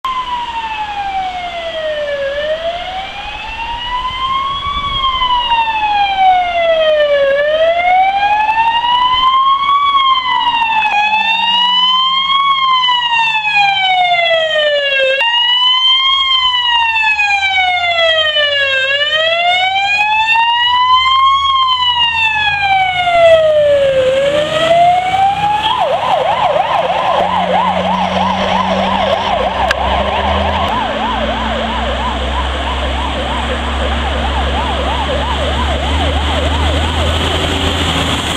315[W] 22 -- Alarmowo 23.02.2012 -- Warszawa
Przejazd alarmowy od jednego zdarzenia do drugiego. Z kierunku Osiedla Bródno w Warszawie na kierunek miejscowości Ząbki w powiecie Wołomińskim. Nagranie wykonano ubiegłej nocy około godziny 01:00. Pozdrawiam!